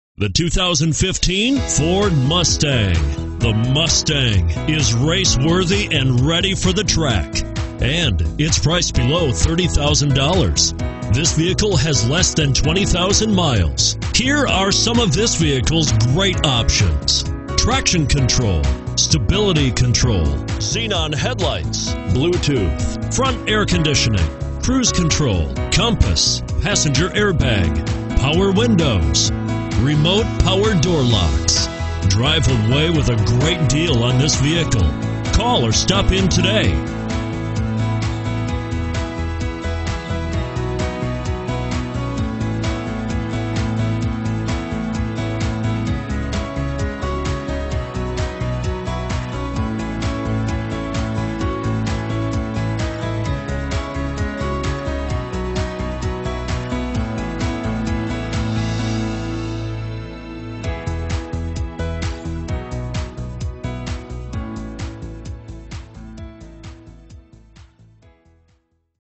2015 Ford Mustang DC, Maryland, and Virginia 94843
2015 Ford Mustang V6 - $26995

Easterns of Baltimore

Looking for the right ? Today could be your lucky day. With 15794 miles, this Grey, 2015 Ford Mustang equipped with Automatic transmission could be yours. Request more information and set up a test drive right away.